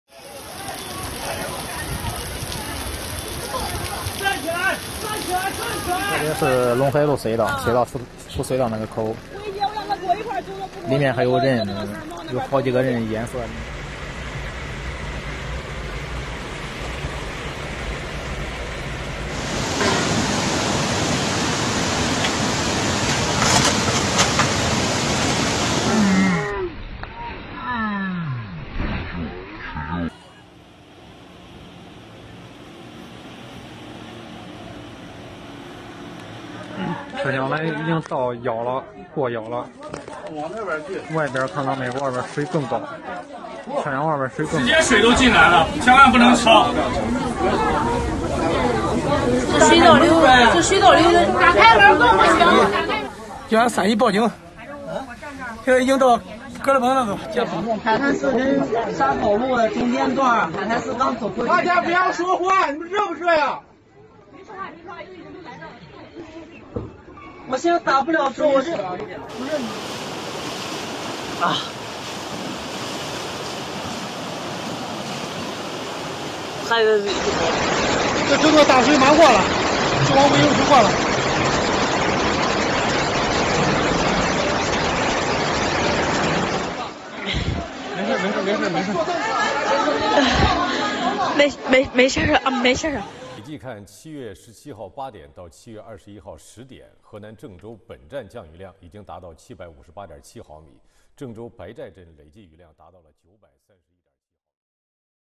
Chuvas intensas causam 12 mortes e 200 mil deslocados na China. Veja as imagens dos estragos
Quase 200 mil pessoas foram retiradas de Zhengzhou, a cidade do centro da China atingida por enchentes que fizeram pelo menos 12 mortos e milhares de desalojados. A evacuação da cidade, que conta mais de 10 milhões de habitantes, ocorreu após as chuvas torrenciais de terça-feira terem deixado parte da rede de metropolitano submersa e terem transformado ruas em canais com um rápido fluxo de água.